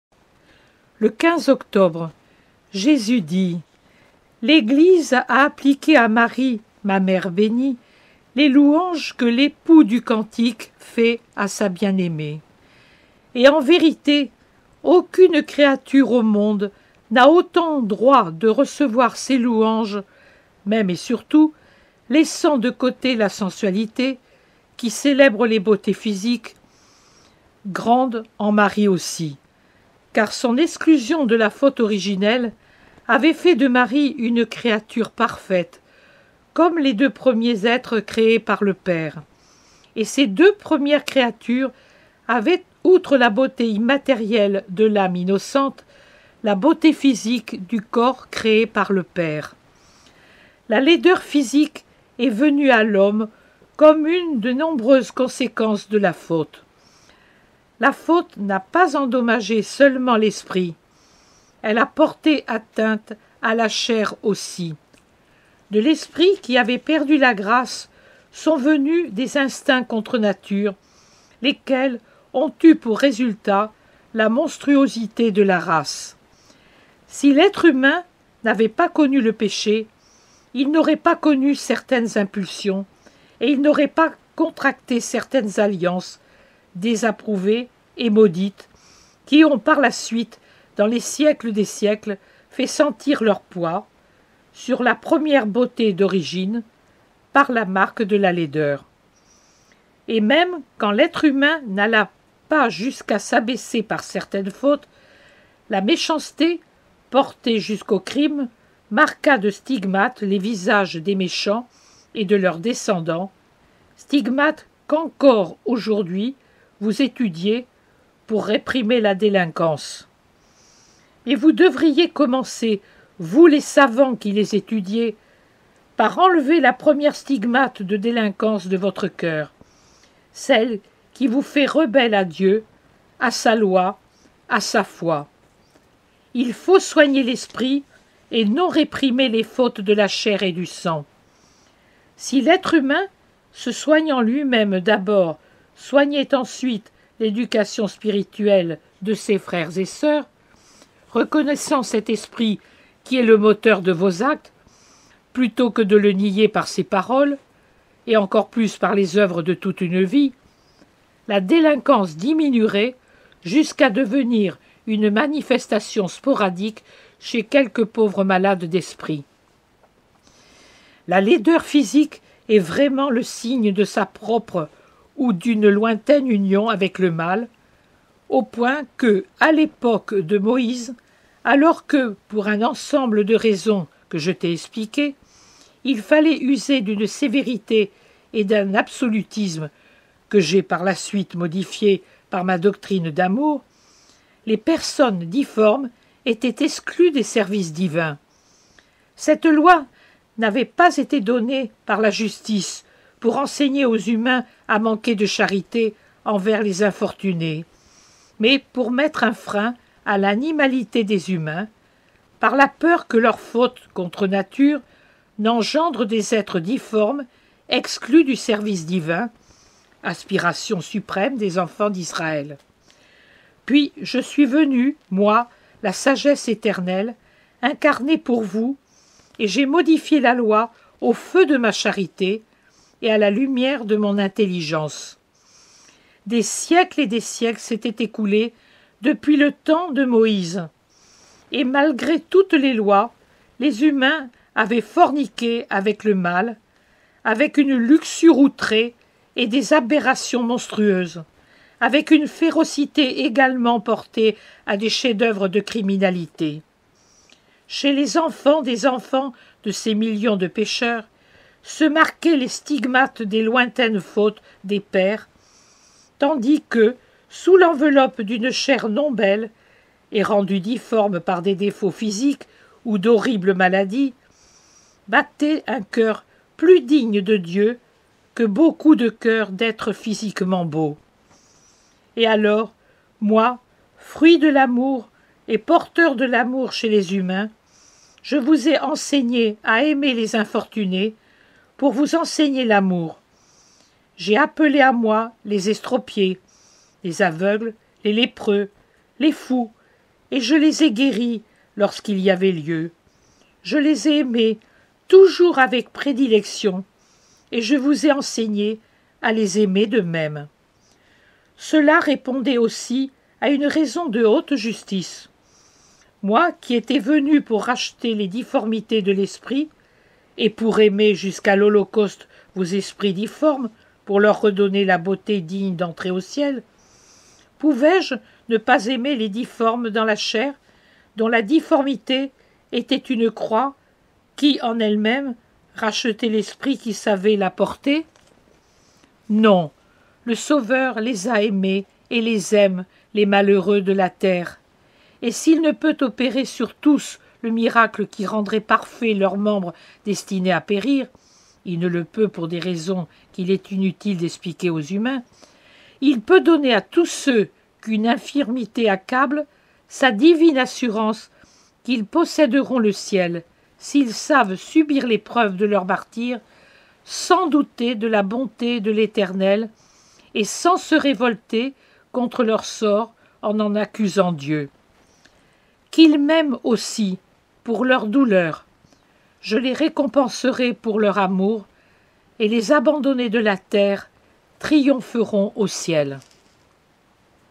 15 Octobre 1943 : A – 𝐋𝐞𝐬 𝐥𝐨𝐮𝐚𝐧𝐠𝐞𝐬 𝐝𝐞 𝐥’𝐞𝐩𝐨𝐮𝐱 à 𝐬𝐚 𝐛𝐢𝐞𝐧-𝐚𝐢𝐦𝐞𝐞 𝐚𝐩𝐩𝐥𝐢𝐪𝐮𝐞𝐞𝐬 à 𝐌𝐚𝐫𝐢𝐞.
*𝐂𝐚𝐭é𝐜𝐡è𝐬𝐞 𝐝𝐮 𝐯𝐞𝐧𝐝𝐫𝐞𝐝𝐢 𝟏𝟓 𝐨𝐜𝐭𝐨𝐛𝐫𝐞 𝟏𝟗𝟒𝟑 : 𝐀 – 𝐋𝐞𝐬 𝐥𝐨𝐮𝐚𝐧𝐠𝐞𝐬 𝐝𝐞 𝐥’é𝐩𝐨𝐮𝐱 à 𝐬𝐚 𝐛𝐢𝐞𝐧-𝐚𝐢𝐦é𝐞 𝐚𝐩𝐩𝐥𝐢𝐪𝐮é𝐞𝐬 à 𝐌𝐚𝐫𝐢𝐞. | 𝐋𝐞𝐬 𝐂𝐚𝐡𝐢𝐞𝐫𝐬 𝐝𝐞 𝟏𝟗𝟒𝟑 - 𝐌𝐚𝐫𝐢𝐚 𝐕𝐚𝐥𝐭𝐨𝐫𝐭𝐚*
𝘑é𝘴𝘶𝘴 𝘥𝘪𝘵 : « 𝘓’É𝘨𝘭𝘪𝘴𝘦 𝘢 𝘢𝘱𝘱𝘭𝘪𝘲𝘶é à 𝘔𝘢𝘳𝘪𝘦, 𝘮𝘢 𝘔è𝘳𝘦 𝘣é𝘯𝘪𝘦, 𝘭𝘦𝘴 𝘭𝘰𝘶𝘢𝘯𝘨𝘦𝘴 𝘲𝘶𝘦 𝘭’é𝘱𝘰𝘶𝘹 𝘥𝘶 𝘊𝘢𝘯𝘵𝘪𝘲𝘶𝘦 𝘧𝘢𝘪𝘵 à 𝘴𝘢 𝘣𝘪𝘦𝘯-𝘢𝘪𝘮é𝘦. 𝘌𝘵 𝘦𝘯 𝘷é𝘳𝘪𝘵é, 𝘢𝘶𝘤𝘶𝘯𝘦 𝘤𝘳é𝘢𝘵𝘶𝘳𝘦 𝘢𝘶 𝘮𝘰𝘯𝘥𝘦 𝘯’𝘢 𝘢𝘶𝘵𝘢𝘯𝘵 𝘥𝘳𝘰𝘪𝘵 𝘥𝘦 𝘳𝘦𝘤𝘦𝘷𝘰𝘪𝘳 𝘤𝘦𝘴 𝘭𝘰𝘶𝘢𝘯𝘨𝘦𝘴, 𝘮ê𝘮𝘦 𝘦𝘵 𝘴𝘶𝘳𝘵𝘰𝘶𝘵 𝘭𝘢𝘪𝘴𝘴𝘢𝘯𝘵 𝘥𝘦 𝘤ô𝘵é 𝘭𝘢 𝘴𝘦𝘯𝘴𝘶𝘢𝘭𝘪𝘵é 𝘲𝘶𝘪 𝘤é𝘭è𝘣𝘳𝘦 𝘭𝘦𝘴 𝘣𝘦𝘢𝘶𝘵é𝘴 𝘱𝘩𝘺𝘴𝘪𝘲𝘶𝘦𝘴, 𝘨𝘳𝘢𝘯𝘥𝘦𝘴 𝘦𝘯 𝘔𝘢𝘳𝘪𝘦 𝘢𝘶𝘴𝘴𝘪, 𝘤𝘢𝘳 𝘴𝘰𝘯 𝘦𝘹𝘤𝘭𝘶𝘴𝘪𝘰𝘯 𝘥𝘦 𝘭𝘢 𝘧𝘢𝘶𝘵𝘦 𝘰𝘳𝘪𝘨𝘪𝘯𝘦𝘭𝘭𝘦 𝘢𝘷𝘢𝘪𝘵 𝘧𝘢𝘪𝘵 𝘥𝘦 𝘔𝘢𝘳𝘪𝘦 𝘶𝘯𝘦 𝘤𝘳é𝘢𝘵𝘶𝘳𝘦 𝘱𝘢𝘳𝘧𝘢𝘪𝘵𝘦 𝘤𝘰𝘮𝘮𝘦 𝘭𝘦𝘴 𝘥𝘦𝘶𝘹 𝘱𝘳𝘦𝘮𝘪𝘦𝘳𝘴 ê𝘵𝘳𝘦𝘴 𝘤𝘳éé𝘴 𝘱𝘢𝘳 𝘭𝘦 𝘗è𝘳𝘦. 𝘌𝘵 𝘤𝘦𝘴 𝘥𝘦𝘶𝘹 𝘱𝘳𝘦𝘮𝘪è𝘳𝘦𝘴 𝘤𝘳é𝘢𝘵𝘶𝘳𝘦𝘴 𝘢𝘷𝘢𝘪𝘦𝘯𝘵, 𝘰𝘶𝘵𝘳𝘦 𝘭𝘢 𝘣𝘦𝘢𝘶𝘵é 𝘪𝘮𝘮𝘢𝘵é𝘳𝘪𝘦𝘭𝘭𝘦 𝘥𝘦 𝘭’â𝘮𝘦 𝘪𝘯𝘯𝘰𝘤𝘦𝘯𝘵𝘦, 𝘭𝘢 𝘣𝘦𝘢𝘶𝘵é 𝘱𝘩𝘺𝘴𝘪𝘲𝘶𝘦 𝘥𝘶 𝘤𝘰𝘳𝘱𝘴 𝘤𝘳éé 𝘱𝘢𝘳 𝘭𝘦 𝘗è𝘳𝘦 [...].»

𝐋𝐞 𝐒𝐞𝐧𝐭𝐢𝐞𝐫 𝐝𝐞 𝐥𝐚 𝐂𝐫𝐨𝐢𝐱 𝐆𝐥𝐨𝐫𝐢𝐞𝐮𝐬𝐞 𝐧𝐞 𝐯𝐢𝐭 𝐪𝐮𝐞 𝐝𝐮 𝐬𝐨𝐮𝐭𝐢𝐞𝐧 𝐝𝐞 𝐬𝐞𝐬 𝐝𝐨𝐧𝐚𝐭𝐞𝐮𝐫𝐬 !

📖 Lu par Anne-Marie Ducourneau